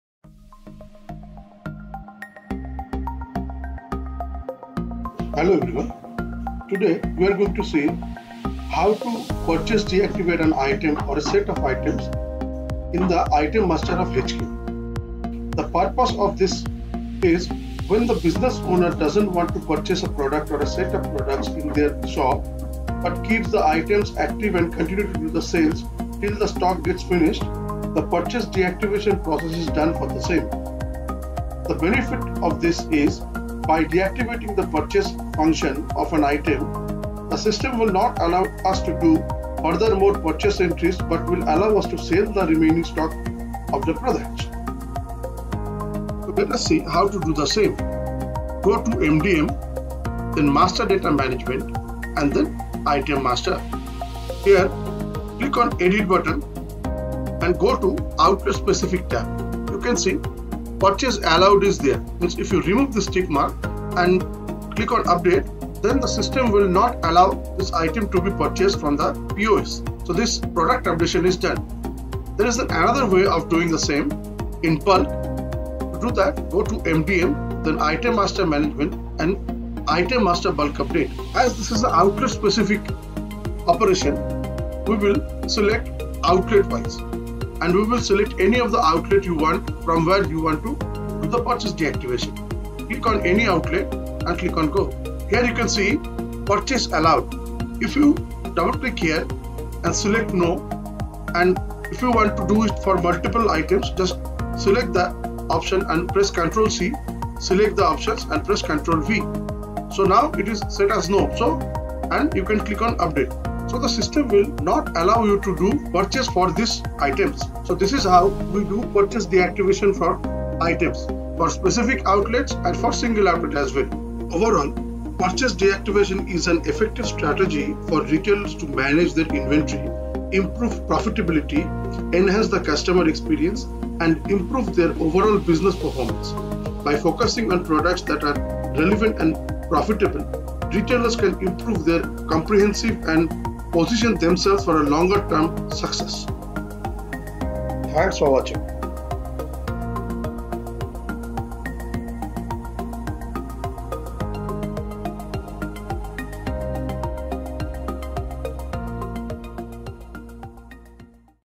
How to Purchase or Deactivate an Item or Set of Items in the Item Master for HQ.
Meaning of Purchase Deactivate:
Purchase deactivation is a strategy used by retailers to temporarily or permanently stop ordering or stocking an item in their store. It can be done for a variety of reasons, such as low demand, poor sales performance, product obsolescence, or high costs.

When a retailer deactivates a product, it means they will not be purchasing any more inventory of that particular item. This can be done in different ways, such as by removing the item from the store shelves, marking it as discontinued or out of stock, or removing it from their ordering system.

Temporary deactivation is often used when a product is experiencing slow sales or when a new model or version of the product is being released. In this case, the retailer may stop ordering the item for a period of time, allowing the existing inventory to sell through before restocking it.

On the other hand, permanent deactivation is used when a product is no longer profitable or relevant to the store's target market. In this case, the retailer may remove the item from their inventory completely, either by returning unsold stock to the supplier or liquidating it through discount sales.

Overall, purchase deactivation is a common practice in the retail industry, used to manage inventory and ensure that retailers are stocking products that are profitable and relevant to their customers.

Benefits:
The benefits of purchase deactivation for an item in a retail business include:

Cost savings: By deactivating products that are not selling well or are no longer profitable, retailers can reduce inventory costs, avoid overstocking, and focus their resources on products that are more likely to generate revenue.

Improved cash flow: By reducing inventory costs, retailers can free up cash flow that can be used for other purposes, such as investing in new products or improving their store operations.

Improved inventory management: Purchase deactivation helps retailers to manage their inventory more efficiently by focusing on products that are more likely to sell and reducing the amount of slow-moving or obsolete inventory.

Enhanced customer satisfaction: By focusing on products that are relevant to their customers, retailers can enhance customer satisfaction and loyalty, leading to repeat business and positive word-of-mouth.

Increased sales: By focusing on products that are more likely to sell, retailers can increase their sales and revenue, leading to improved business performance.

Improved store operations: By reducing the number of products they stock, retailers can simplify their store operations, making it easier to manage their inventory, restock shelves, and fulfill customer orders.

Overall, purchase deactivation can be a powerful tool for retailers to improve their inventory management, reduce costs, enhance customer satisfaction, and increase sales. It enables retailers to focus their resources on products that are more likely to generate revenue and improve their overall business performance.